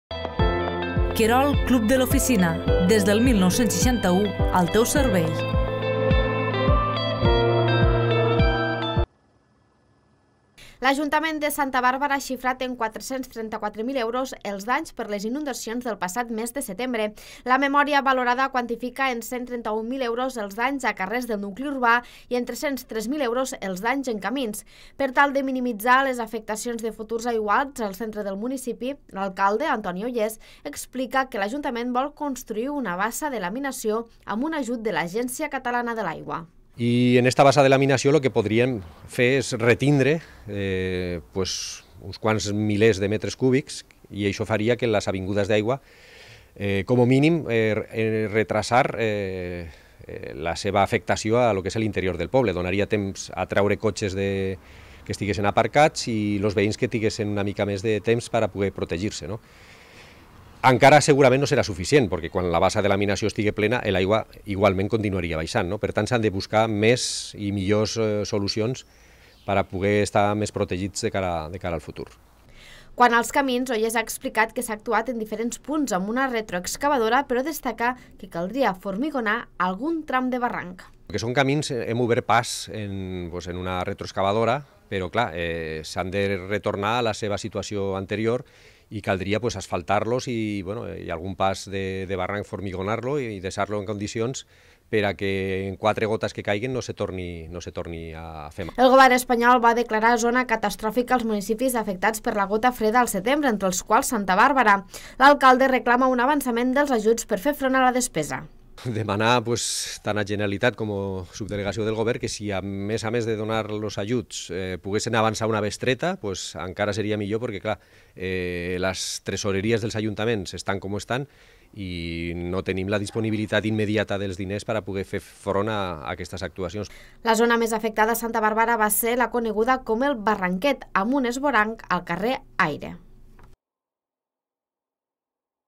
Santa Bàrbara vol construir una bassa de laminació per minimitzar l’efecte dels aiguats
L’Ajuntament de Santa Bàrbara ha xifrat en 434.000 euros els danys per les inundacions del passat mes de setembre. La memòria valorada quantifica en 131.000 euros els danys a carrers del nucli urbà i en 303.000 euros els danys en camins. Per tal de minimitzar les afectacions de futurs aiguats al centre del municipi, l’alcalde, Antonio Ollés, explica que l’Ajuntament vol construir una bassa de laminació amb un ajut de l’Agència Catalana de l’Aigua.

Quant als camins, Ollés ha explicat que s’ha actuat en diferents punts amb una retroexcavadora però destaca que caldria formigonar algun tram de barranc.

El govern espanyol va declarar zona catastròfica els municipis afectats per la gota freda al setembre, entre els quals Santa Bàrbara. L’alcalde reclama un avançament dels ajuts per fer front a la despesa.

La zona més afectada a Santa Bàrbara va ser la coneguda com el Barranquet, amb un esvoranc al carrer Aire.